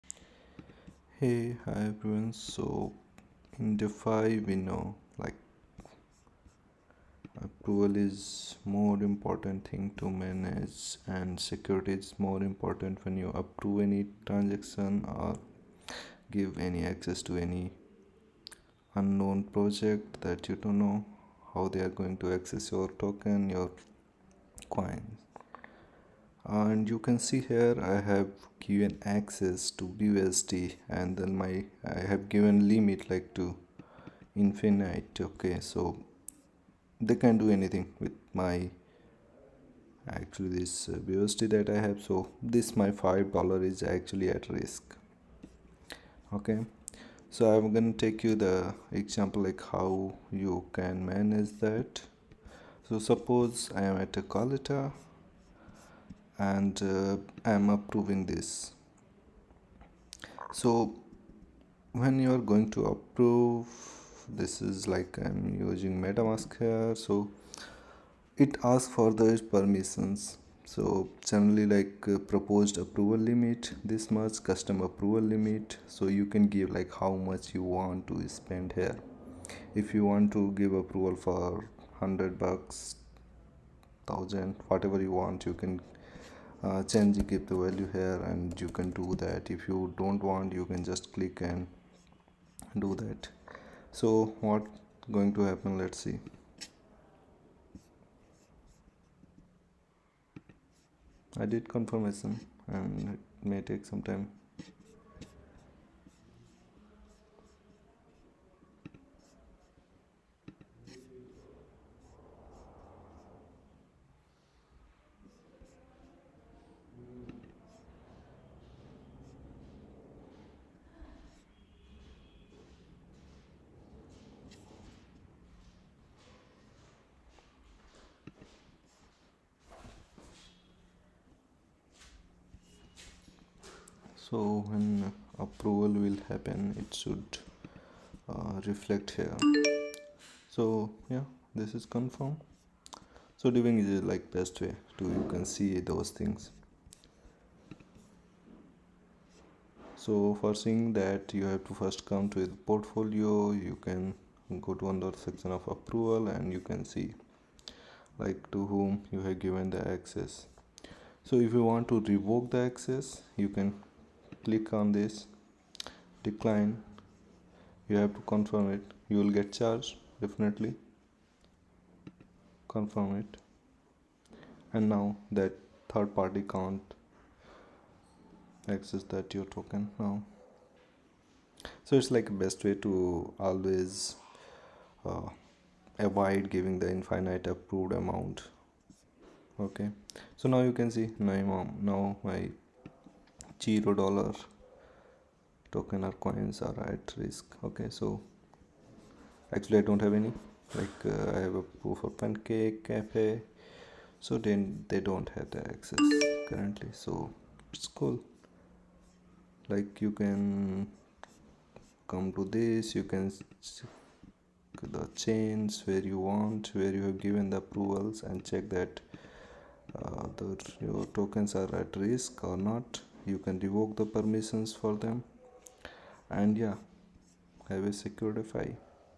MetaMask Token Approval to Secure Wallet
Manage MetaMask token approvals to secure your wallet
How to revoke approvals ?
Avoid risk of PROXY wallet access !
Using